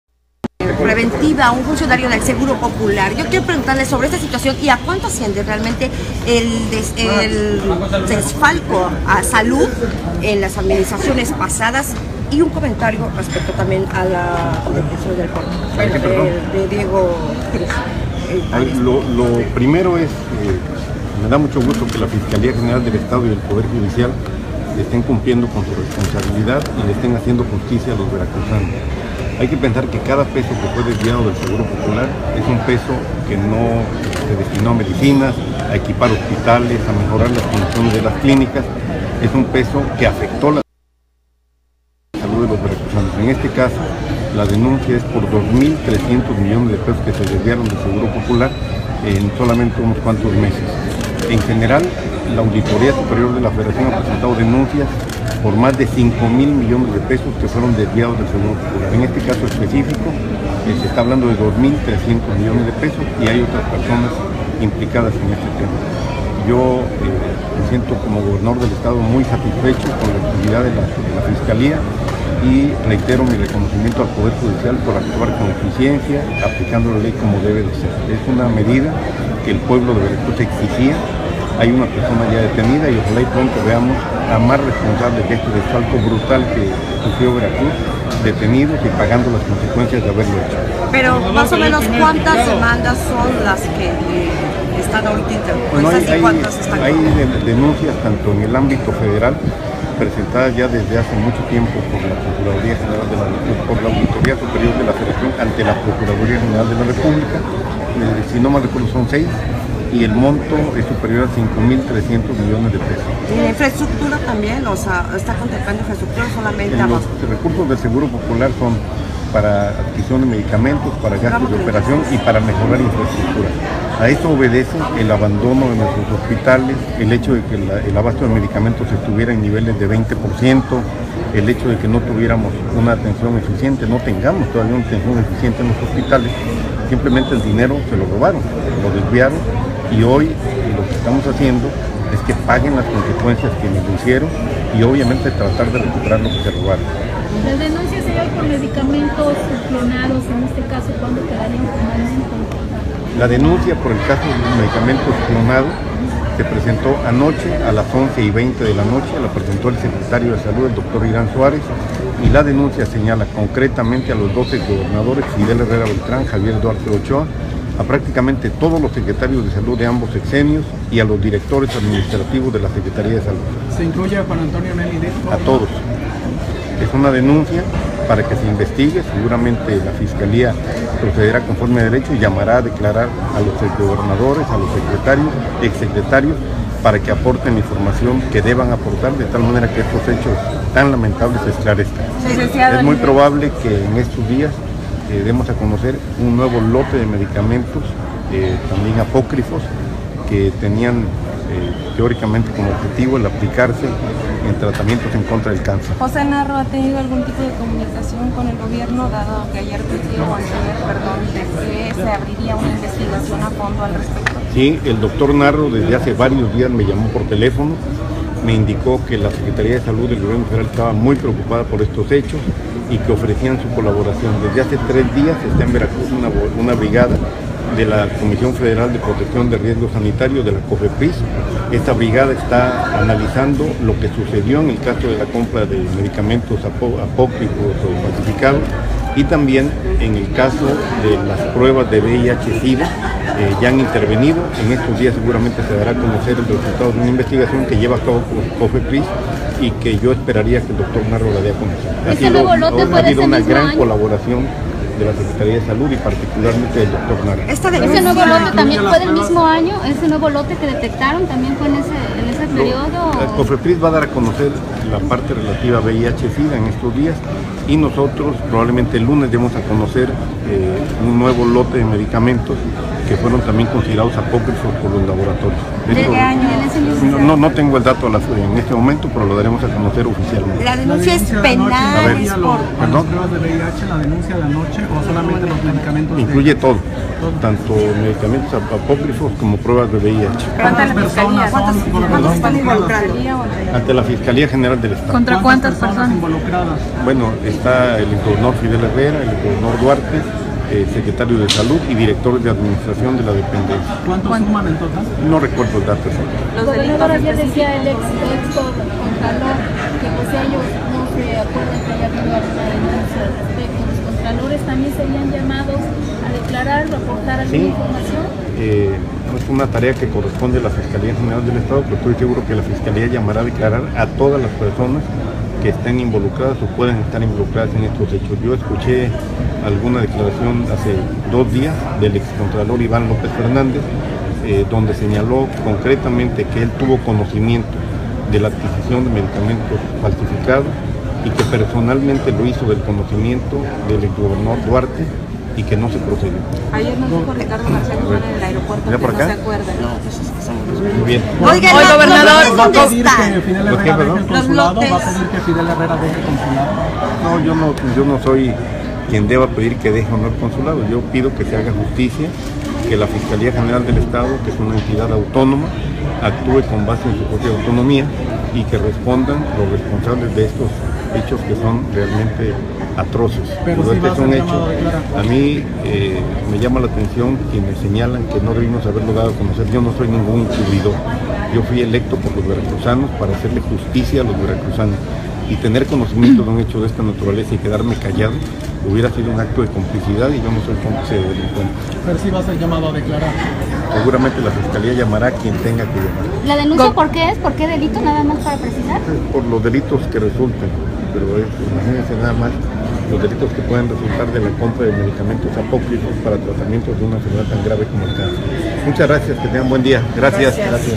Gob Yunes denuncia formalmente a Fidel Herrera y Javier Duarte (entrevista)
**NOTICIERO EN VIVO canal de youtube LUNES, MIÉRCOLES y VIERNES 11:15AM**

**CANAL YOUTUBE